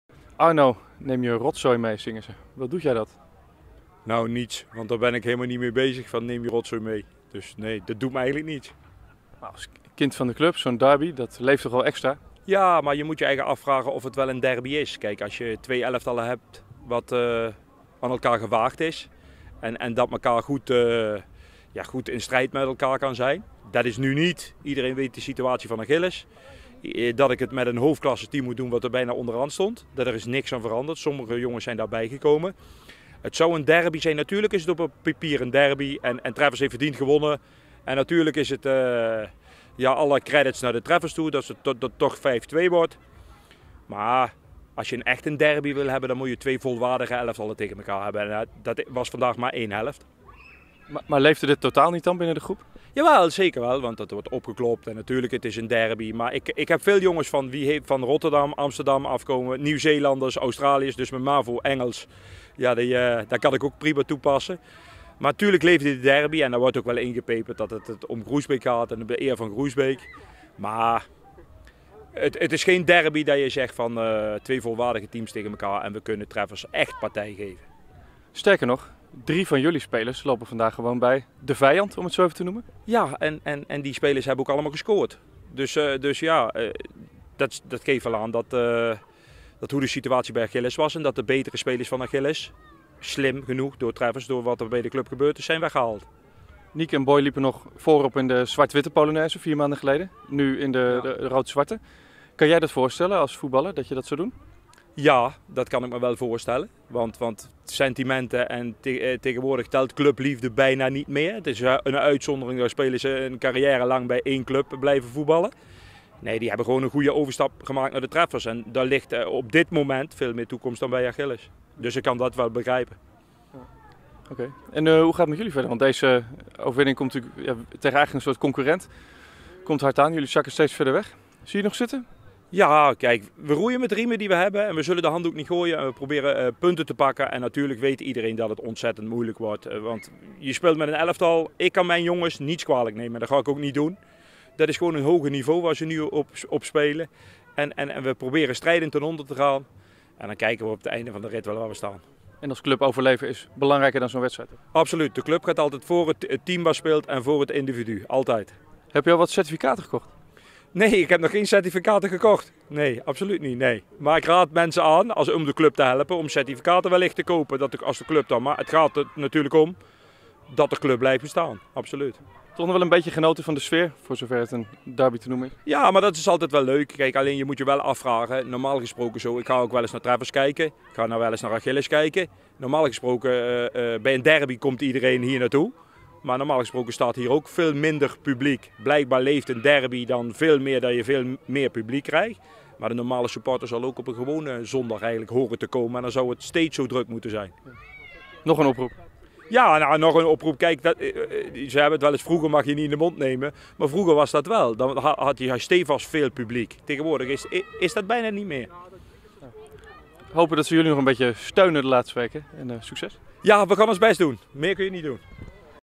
Arts: 'Voor een volwaardigde derby heb je twee gelijke elftallen nodig'
Achilles '29 trainer Arno Arts erkende na afloop van de nederlaag tegen De Treffers (5-2) dat het verschil in niveau erg groot was.